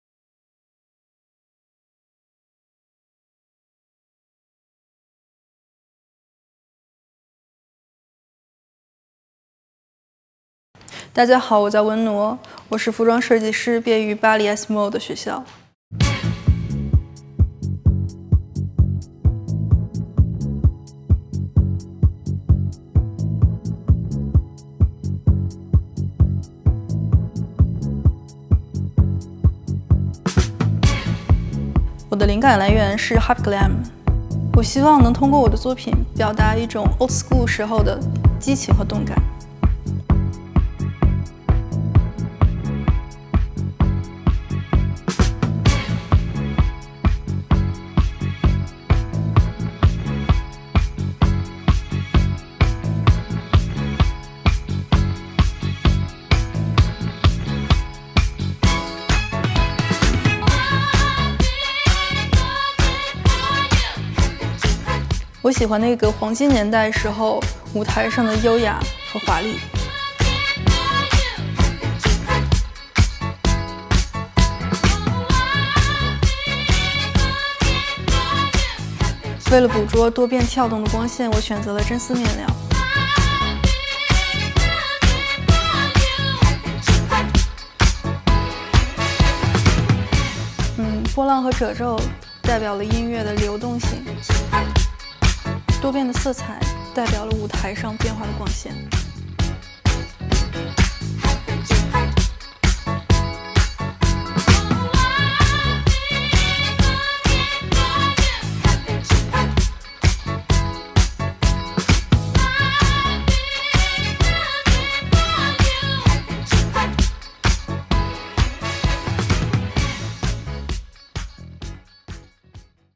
ESMOD X Nelly Rodi - Hype Glam
NellyRodi / Nelly Rodi a fait appel à 4 étudiants d'ESMOD Paris pour illustrer les tendances SS2020 au Salon Intertextile de Shanghai en mars dernier.
Tendance 4 : HYPE GLAM par Wennuo Wang
Vidéo : Fabien Lamotte